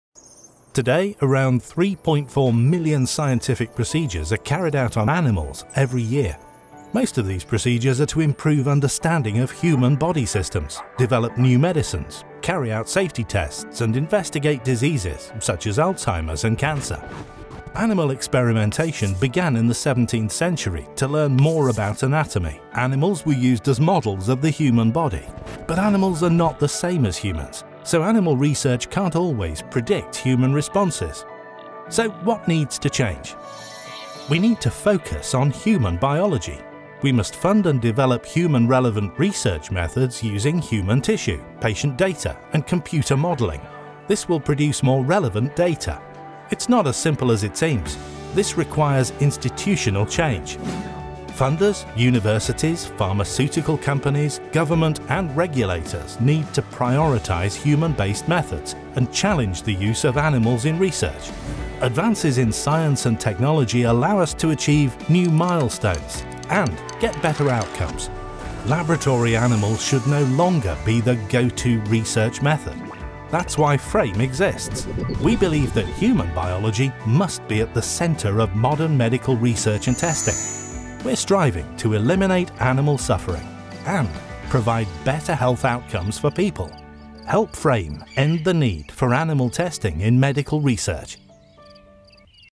FRAME Animation
Today, around 3.4 million scientific procedures are carried out on animals every year. Animal experimentation began in the 17th Century to learn more about anatomy. Animals were used as models of the human body. But animals are not the same as humans so animal research cannot always predict human responses. So what needs to change? Find out more about the work FRAME is doing and our vision for a future without animal testing in this animated video. A special thanks to HelpFilm for creating the wonderful animations.